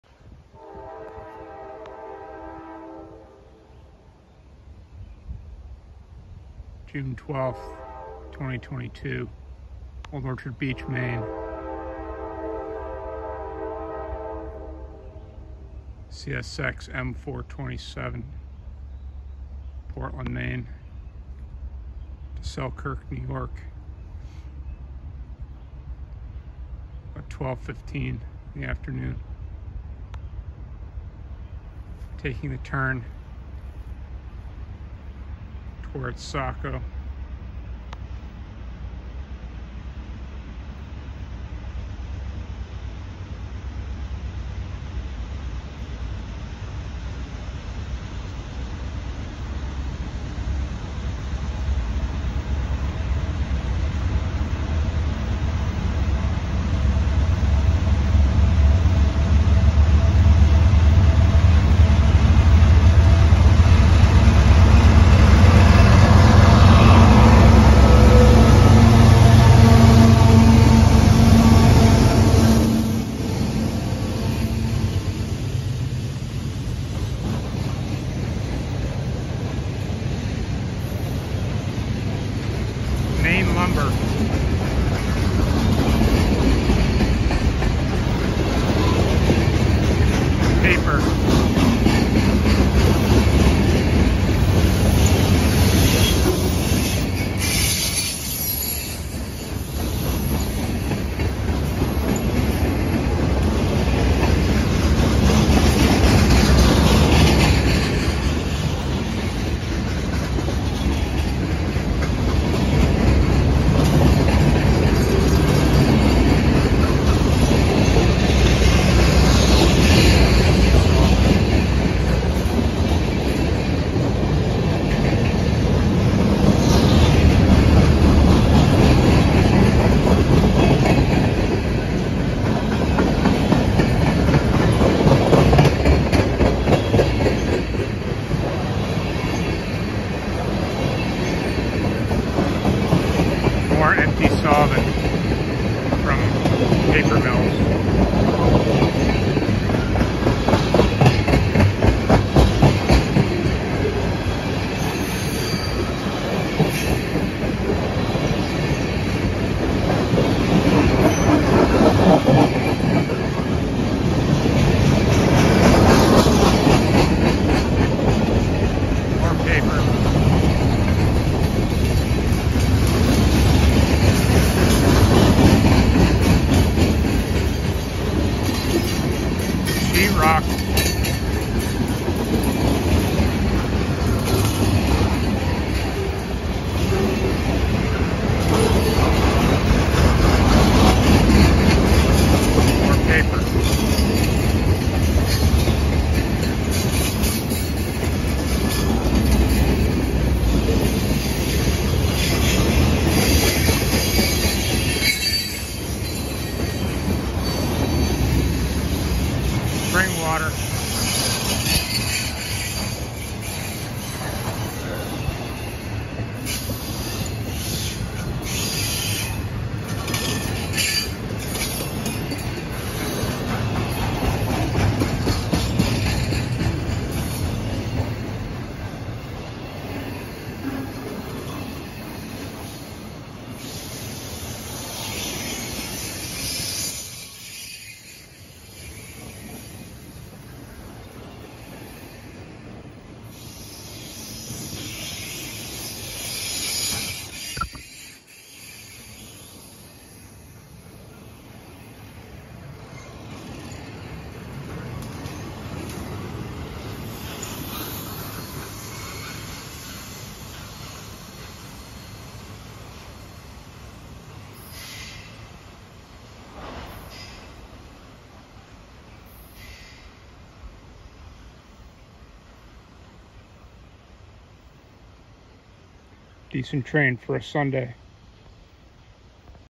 CSX M427 makes the turn towards Saco ME on Sunday 6/12/22
CSX M427 at Old Orchard Beach ME on Sunday 6/12/22